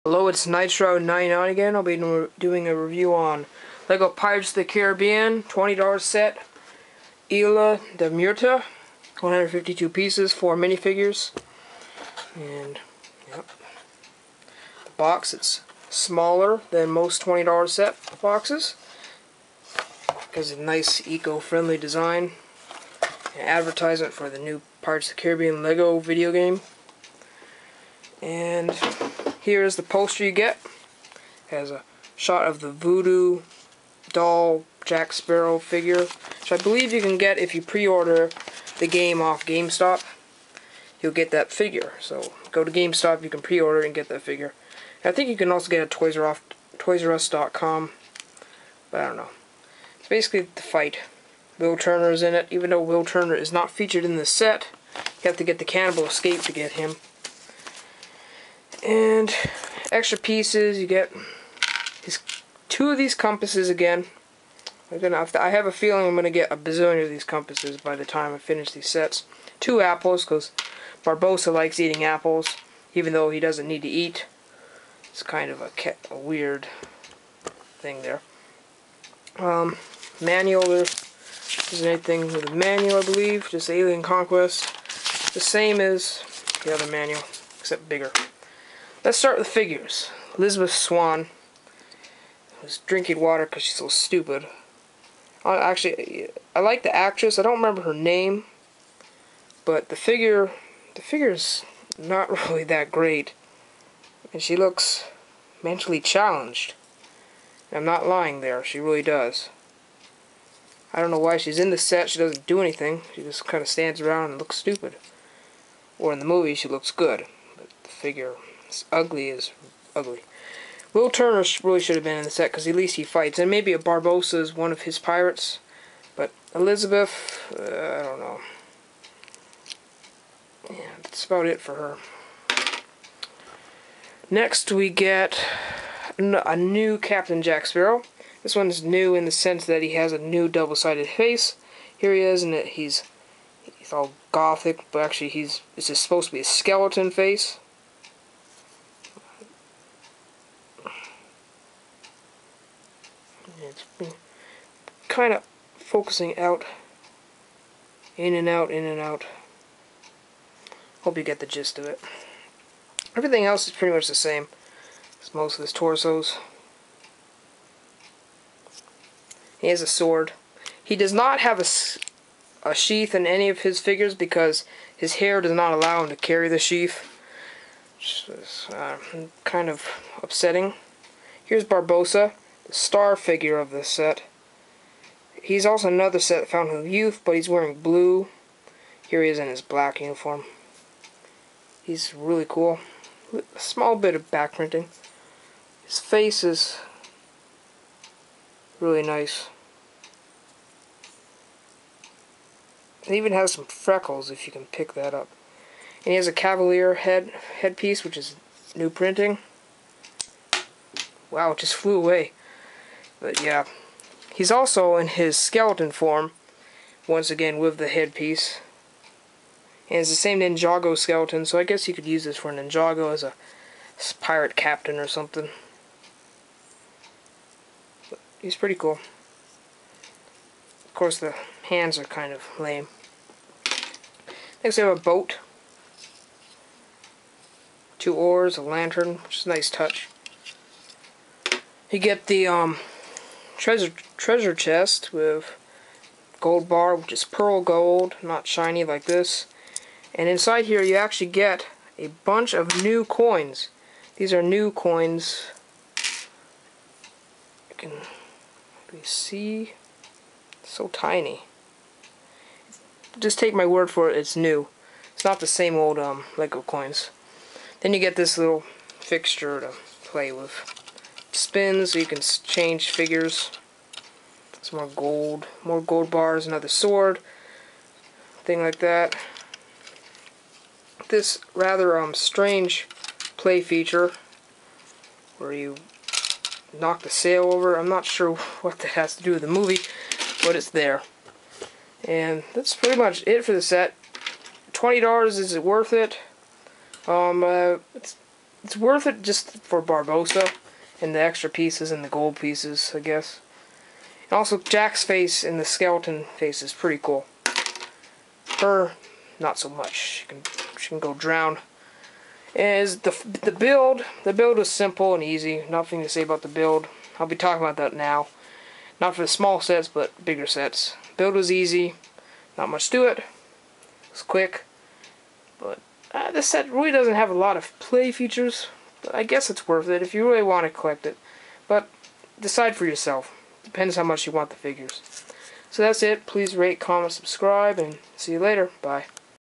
LEGO PIRATES of the CARIBBEAN Isla De Muerta REVIEW:Set 4181
152 pieces
$20 U.S. dollars
4 Minifigures